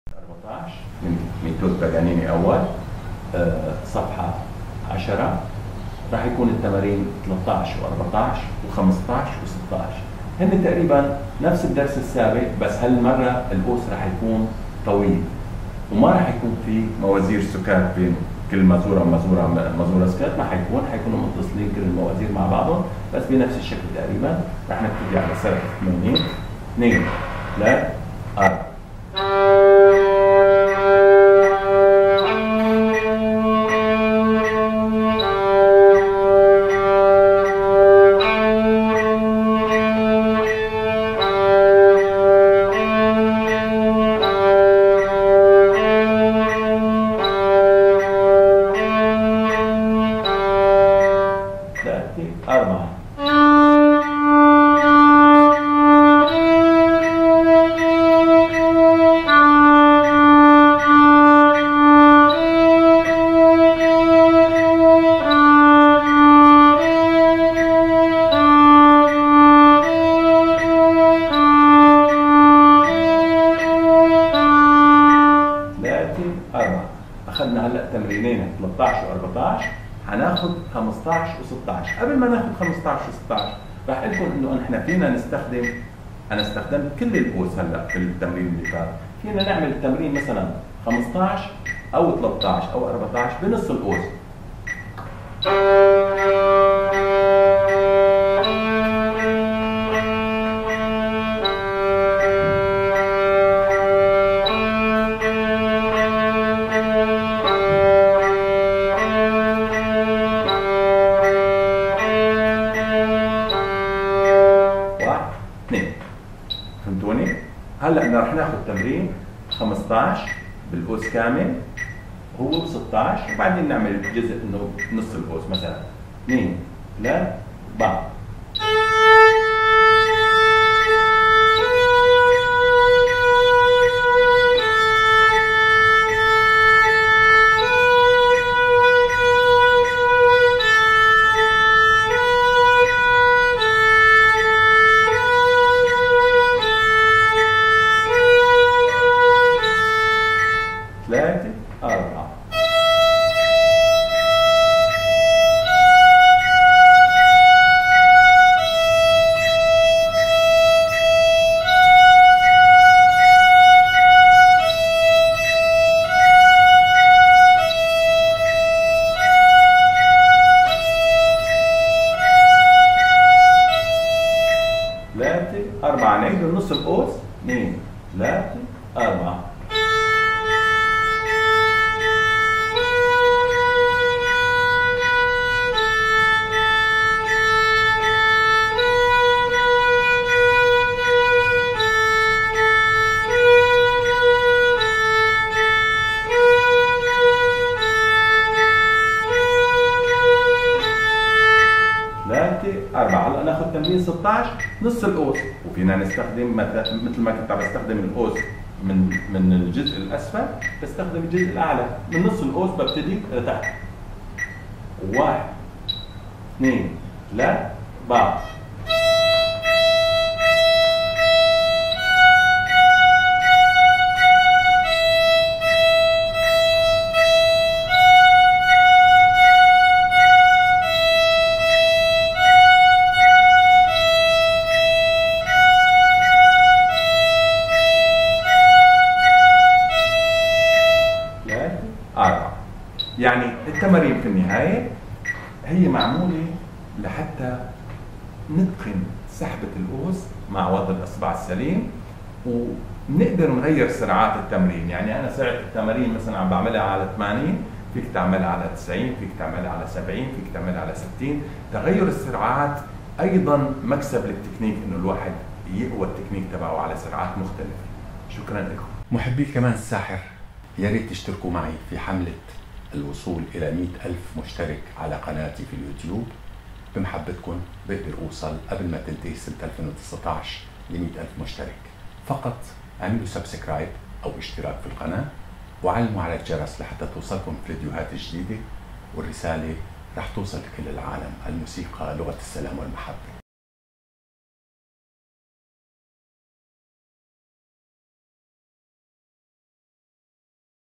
تعليم الكمان 14 Paganini 1
الدرس الرابع عشر من تعليم الكمان الغربي على ميتود باغانيني 1